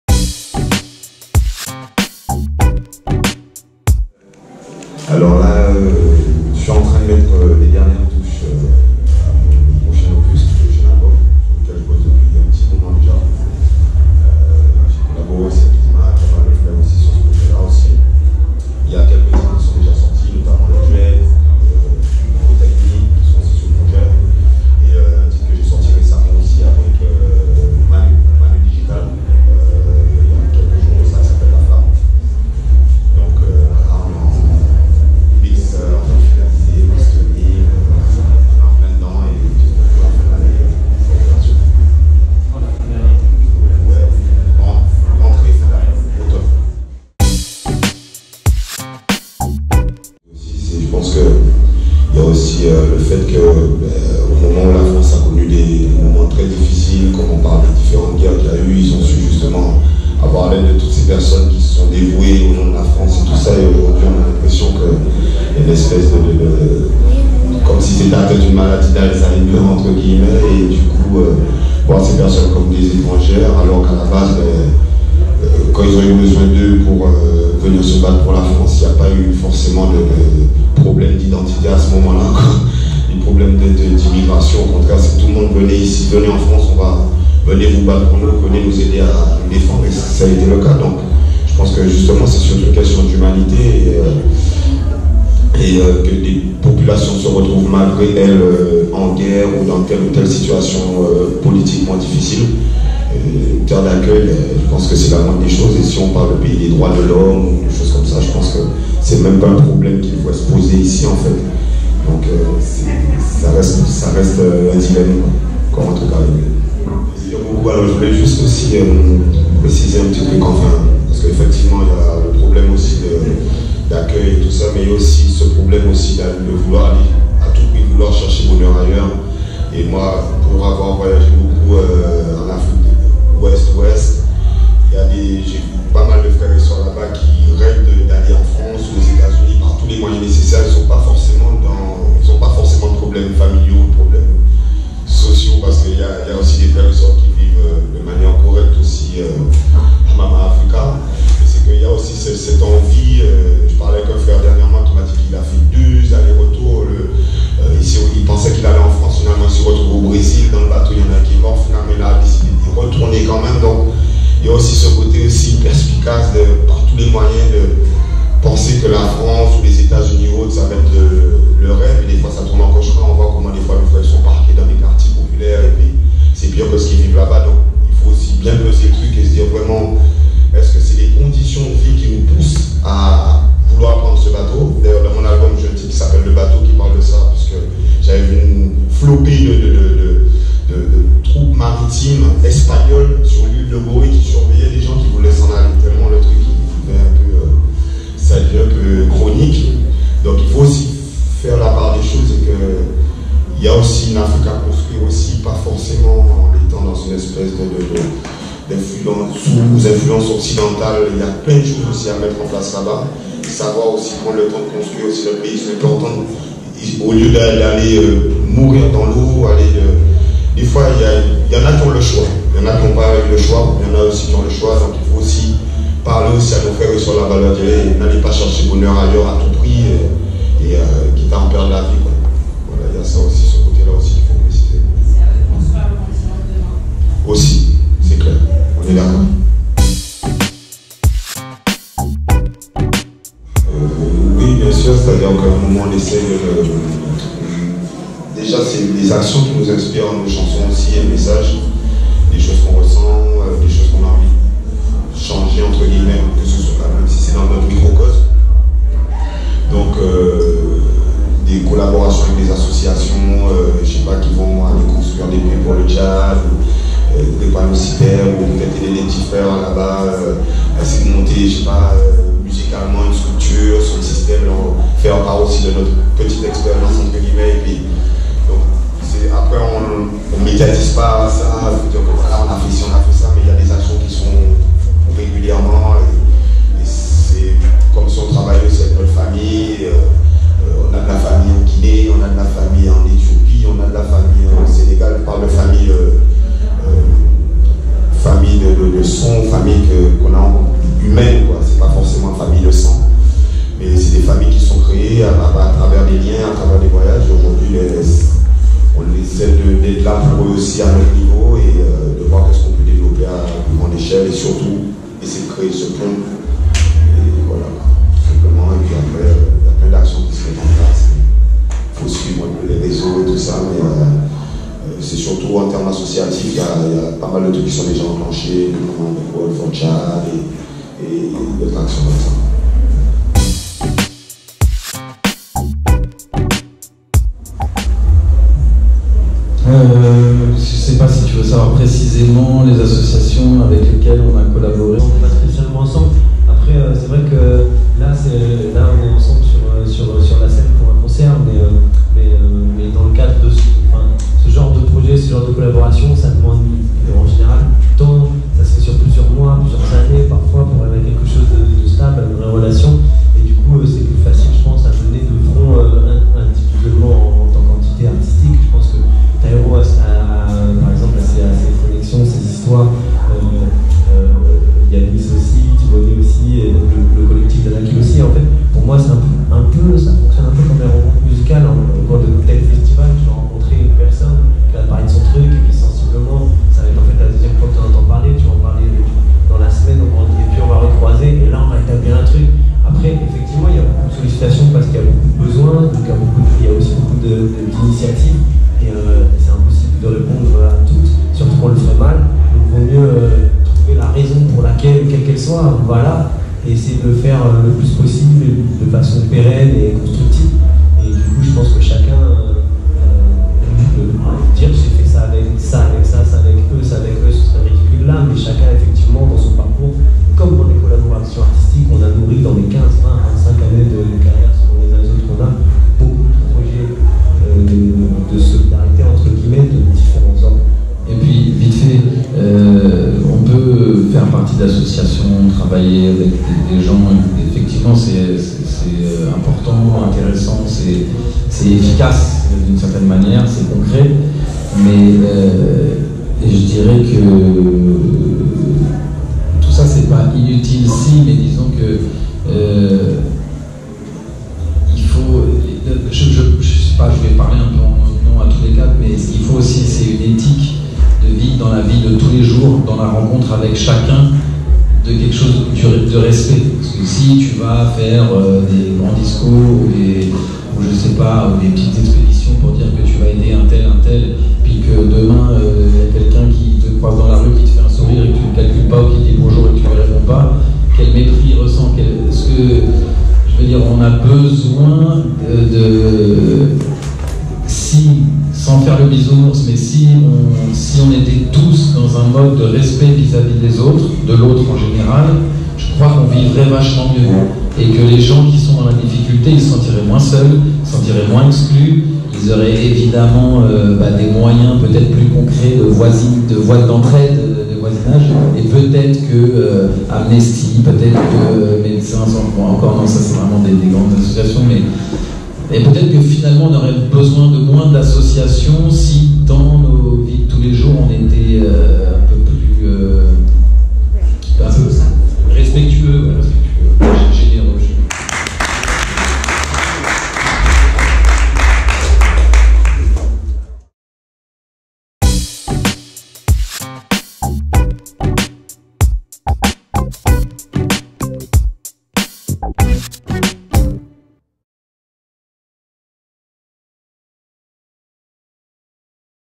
ITW @TIWONYOFFICIAL by La Grosse Radio Reggae & KIKA
Chacun apporte ses réponses aux questions, lors de la conférence de presse du reggae Sun Ska 2K19. Voici celles de ronny.